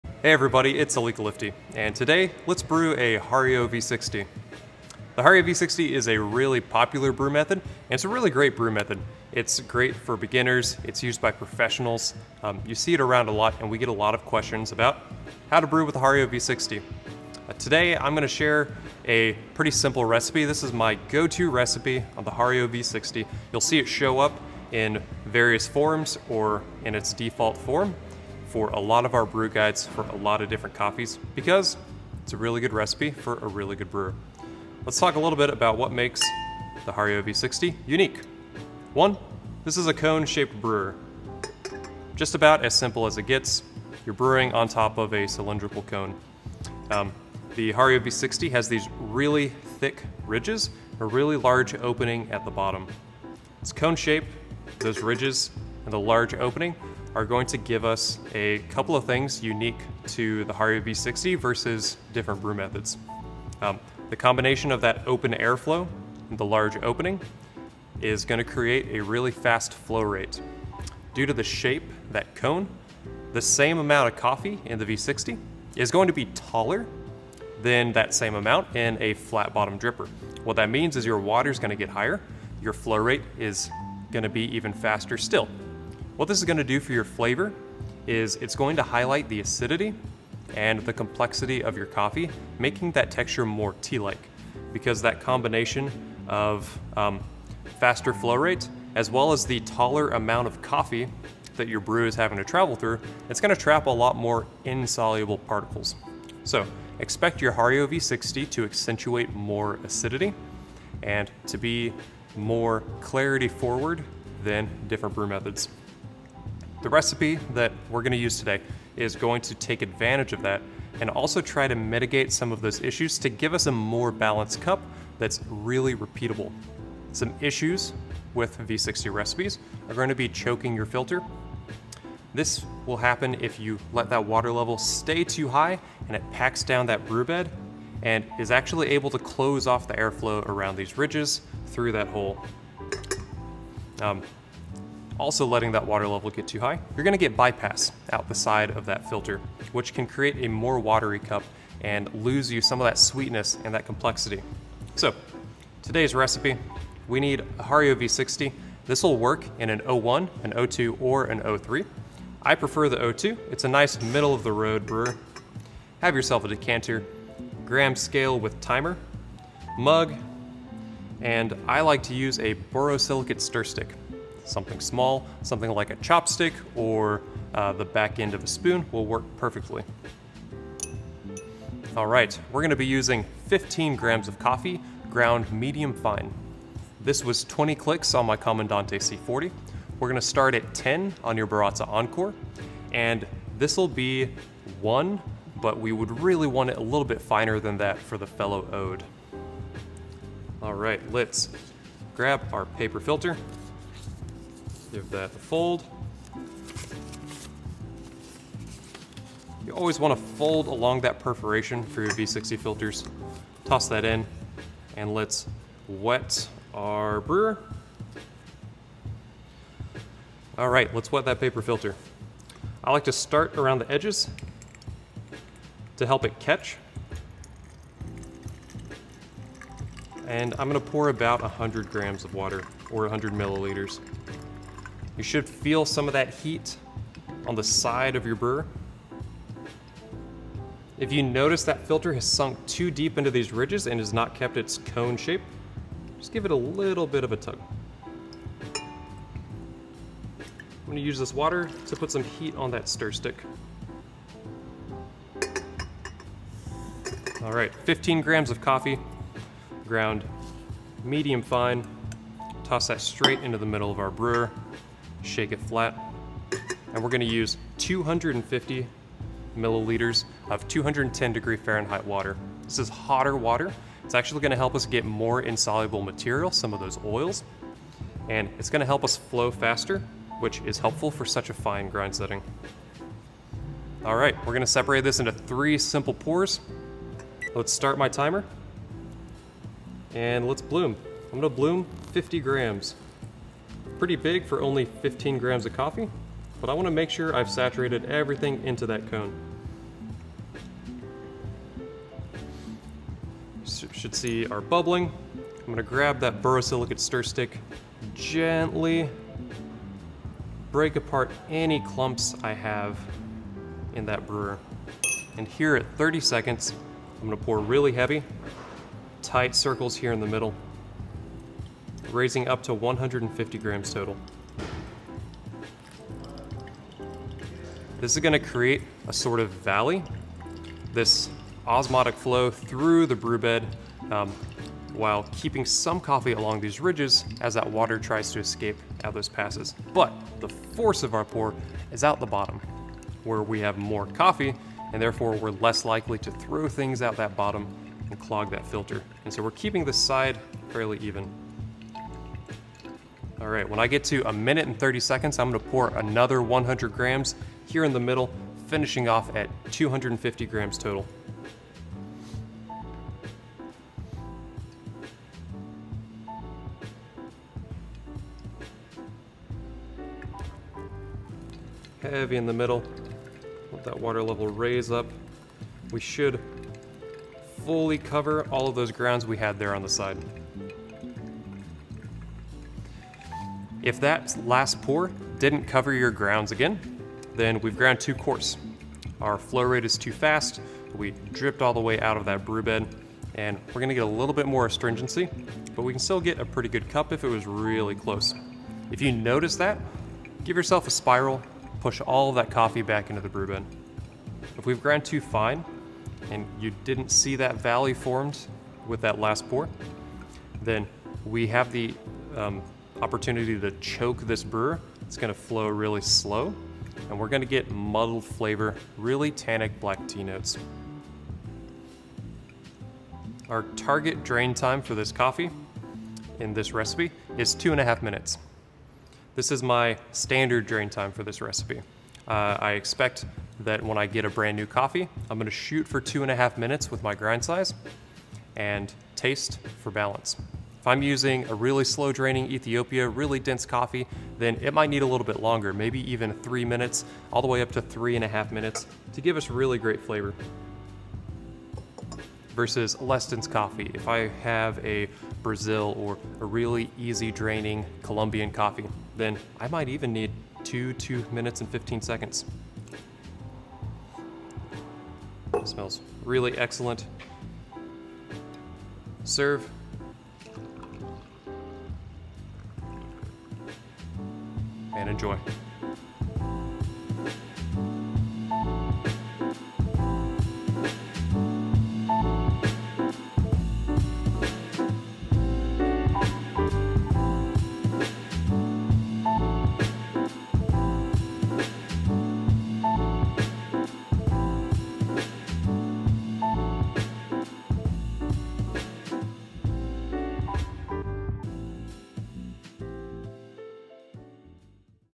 Onyx Coffee Lab - Hario V60 Brewguide
Join US Brewers Cup Champion, Elika, for our go-to Hario V60 recipe and brew-along. This recipe is a simple and fast 3 pours to an excellent and consistent cup of coffee. While somewhat contrarian to other popular approaches to this brewer, you should notice consistent drain times day-to-day.

FILTER – Hario V60
15g Coffee : 250g Water 210°F
Med-fine
10 Baratza Encore
20clix C40
1 Ode

Start Timer - Bloom to 50g
30s - Center pour to 150g
1m30s - Center pour to 250g
~2m30s drain

Notes: If drain is too quick, a spiral pour will slow it down, but attempt to meet target drain and taste for balance.